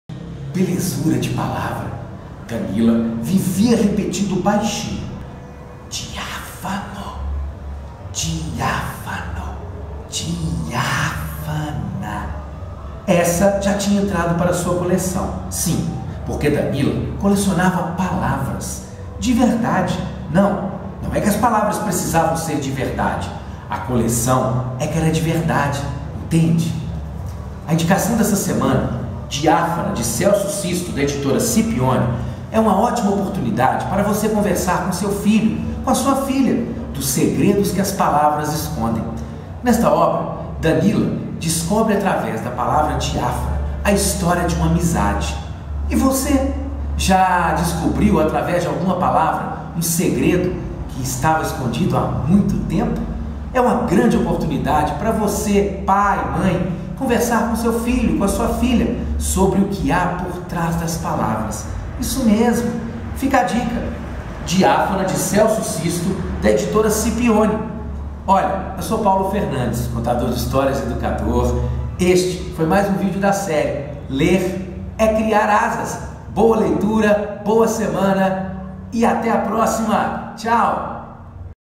Diáfana
Indicação de Leitura do Educador e Contador de Histórias Paulo Fernandes. Parceria Editora Scipione.